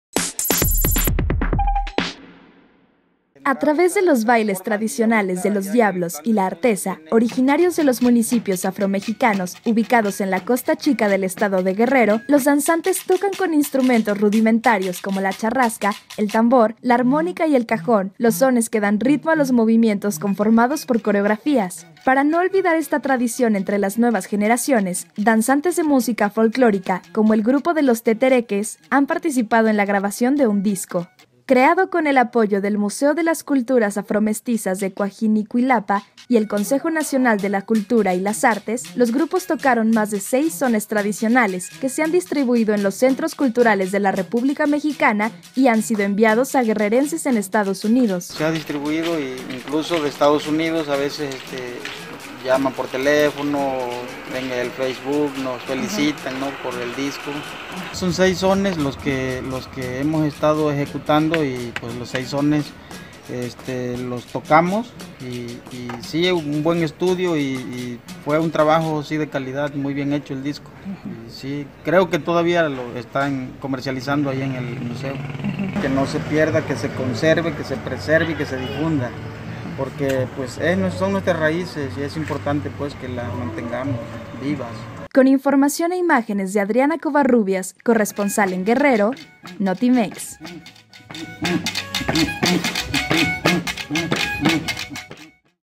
Municipios afromexicanos crean disco con sus danzas tradicionales
Originarios de los municipios afromexicanos de Guerrero, tocan con instrumentos rudimentarios danzas tradicionales